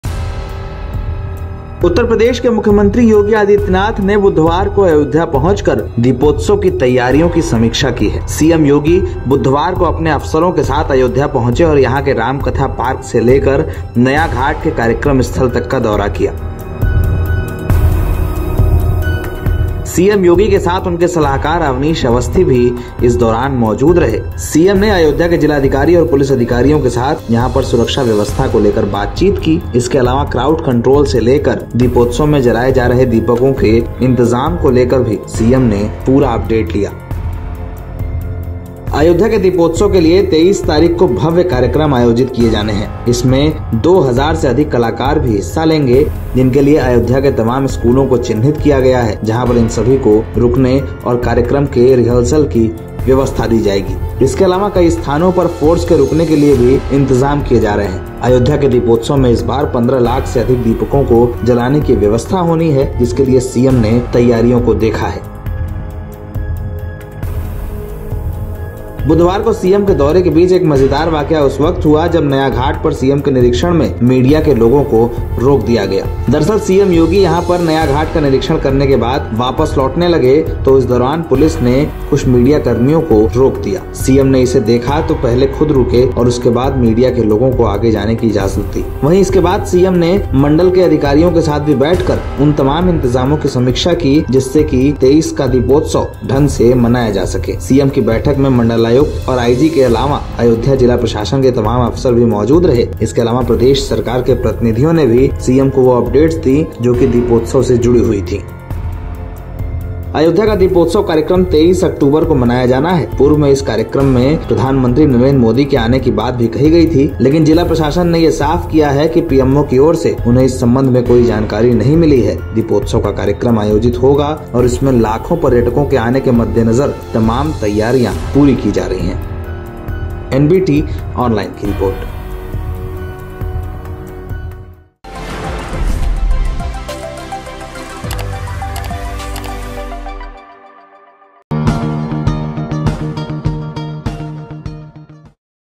Ayodhya Deepotsav 2022 Arrangement देखने पहुंचे CM Yogi, अफसरों संग Foolproof कराई कई चीजें
उत्तर प्रदेश के मुख्यमंत्री योगी आदित्यनाथ ने बुधवार को अयोध्या जिले का दौरा किया और यहां पर दीपोत्सव की तैयारियां देखी। योगी अपने राजकीय हेलीकॉप्टर से अयोध्या पहुंचे और यहां पर अफसरों के साथ दीपोत्सव की तैयारियों का जायजा लिया। योगी ने यहां पर राम कथा पार्क और नया घाट का दौरा भी किया। अयोध्या में 23 अक्टूबर को दीपोत्सव मनाया जाएगा और यहां पर 1500000 दीपक जलाए जाएंगे।